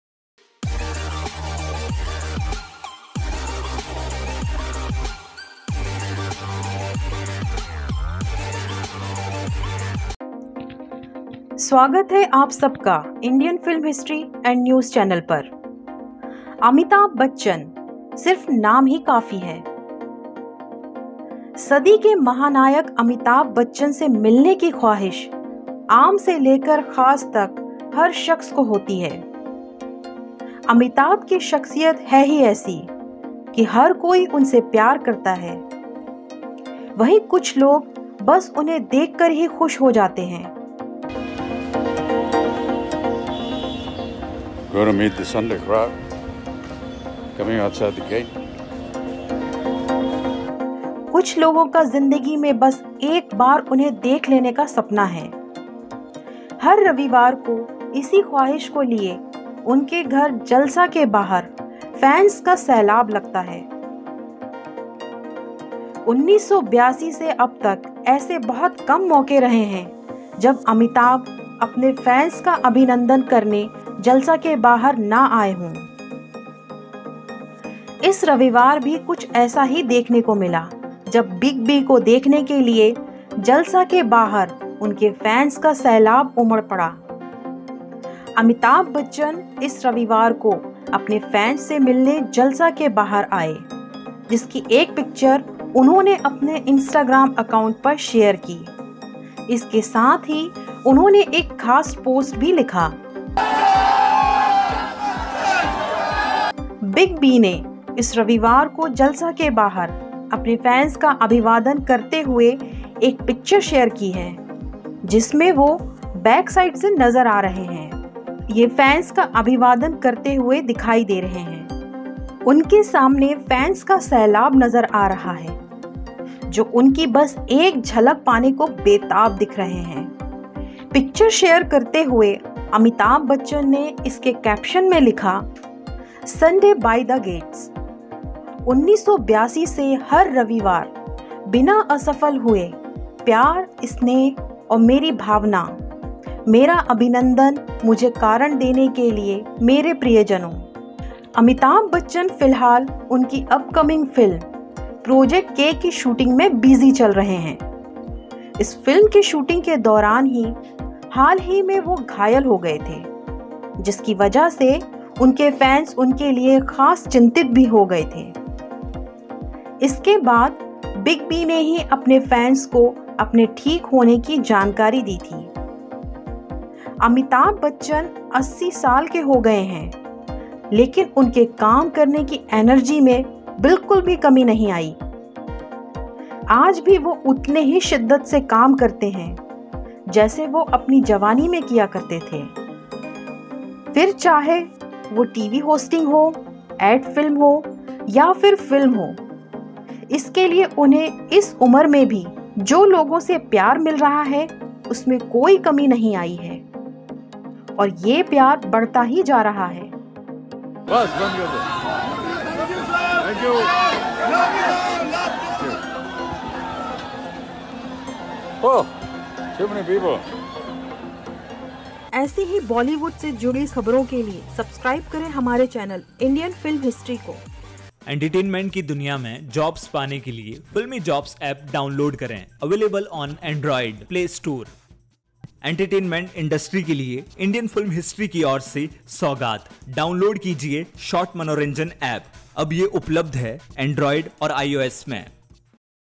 Amitabh's Gets Emotional On Last Sunday Outside Jalsa | Indian Film History
Amitabh Bachchan Only name is enough to meet Saree's megastar Amitabh Bachchan, Amitabh's personality is such that everyone loves him, while some people get happy just seeing him, since 1982 till now there have been very few occasions when Amitabh Amitabh Bachchan came out of Jalsa to meet his fans on this Sunday. He posted the picture on his Instagram account, along with this, he also wrote a special post. Big B has taken a picture of Sher greeting his fans outside Jalsa this Sunday, in which he is seen from the back side, greeting the fans. While watching the picture, Amitabh Bachchan wrote in the caption, "Sunday by the gate every Sunday without fail since 1982, love this one." And my dears, Amitabh Bachchan was recently injured during the shooting of this film, due to which his fans became especially worried for him, after this Big B himself Amitabh Bachchan had informed his fans about his recovery. Amitabh Bachchan has turned 80 years old, but his energy to work has not diminished at all. Even today, he works with the same enthusiasm as he used to do in his youth. Be it TV hosting, ad film or film, the love he is getting from people even at this age has not decreased and this love is increasing.

Address:
1/3,Shere Punjab Colony,
Andheri East.
Mumbai 400 093
India.